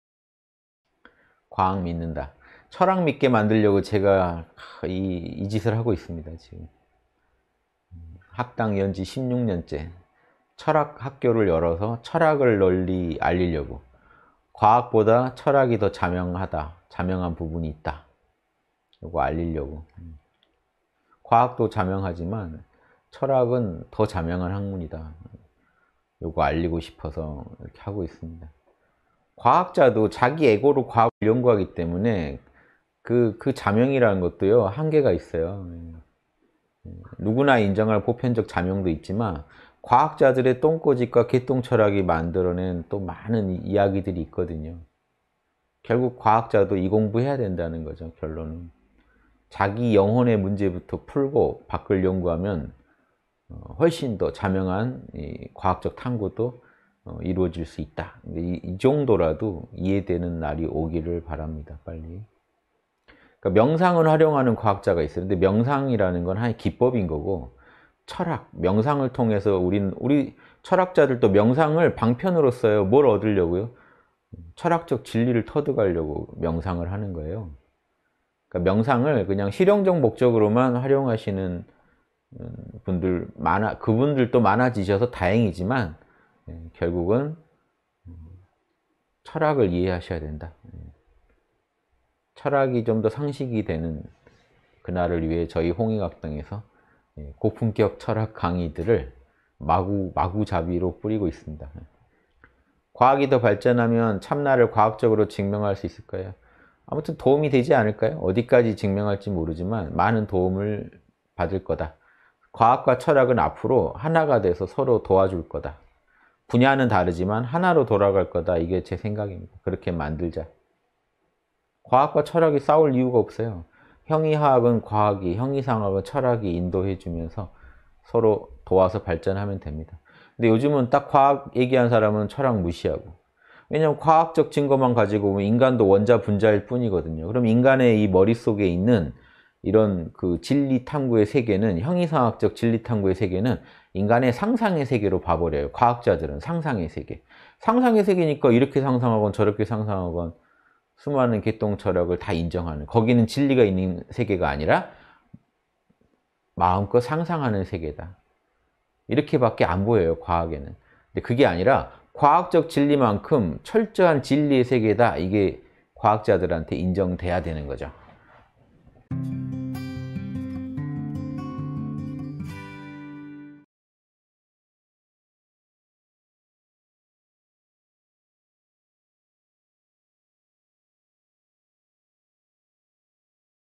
[3분 인문학] 미래는 과학과 철학의 융합시대 _홍익학당
(200523 양심톡톡 토요) ‘과학 믿는다.’ 철학 믿게 만들려고
 제가 이 짓을 하고 있습니다, 지금.
학당 연지 16년째 철학학교를 열어서
철학을 널리 알리려고.
“과학보다 철학이 더 자명하다.
자명한 부분이 있다.” 이거 알리려고.“
과학도 자명하지만, 철학은 더 자명한 학문이다.”
이거 알리고 싶어서 이렇게 하고 있습니다.

과학자도 자기 에고로 과학을 연구하기 때문에
그 자명이라는 것도 한계가 있어요.
누구나 인정할 보편적 자명도 있지만,
과학자들의 똥고집과 개똥철학이 만들어낸
또 많은 이야기들이 있거든요.

결국 과학자도 이 공부해야 된다는 거죠, 결론은.
자기 영혼의 문제부터 풀고,
밖을 연구하면 훨씬 더 자명한 과학적 탐구도 이뤄질 수 있다.’
이 정도라도 이해되는 날이 오기를 바랍니다, 빨리.
명상을 활용하는 과학자가 있어요.
명상이라는 것이 하나의 기법인 거고,
철학, 명상을 통해서,
우리 철학자들도 명상을 방편으로 써요.
“뭘 얻으려고요?”.
철학적 진리를 터득하려고 명상을 하는 거예요.

명상을 그냥 실용적 목적으로만 활용하시는
그분들도 많아지셔서 다행이지만
결국은 철학을 이해하셔야 된다.
철학이 좀 더 상식이 되는 그날을 위해
저희 홍익학당에서 고품격 철학강의들을
마구잡이로 뿌리고 있습니다.

“과학이 더 발전하면 참나를 더 과학적으로 증명할 수 있을까요?”.
아무튼 도움이 되지 않을까요?
‘어디까지 증명할지 모르지만 많은 도움을 받을 거다.
과학과 철학은 앞으로 하나가 돼서 서로 도와줄 거다.
분야는 다르지만 하나로 돌아갈 거다.’
이게 제 생각입니다. 그렇게 만들자.
과학과 철학이 싸울 이유가 없어요.

형이하학은 과학이, 형이상학은 철학이 인도해 주면서
서로 도와서 발전하면 됩니다.
요즘은 과학 얘기하는 사람은 철학 무시하고,
왜냐하면 과학적 증거만 가지고 보면
인간도 원자, 분자일 뿐이거든요.
인간의 이 머릿속에 있는 이런 진리 탐구의 세계는
형이상학적 진리 탐구의 세계는
인간의 상상의 세계로 봐버려요.

과학자들은 상상의 세계.
상상의 세계이니까 이렇게 상상하건 저렇게 상상하건
수많은 개똥철학을 다 인정하는.
‘거기는 진리가 있는 세계가 아니라,
마음껏 상상하는 세계다.’ 이렇게 밖에 안 보여요, 과학에는.
‘그게 아니라 과학적 진리만큼 철저한 진리의 세계다.’
이게 과학자들한테 인정돼야 하는 거죠.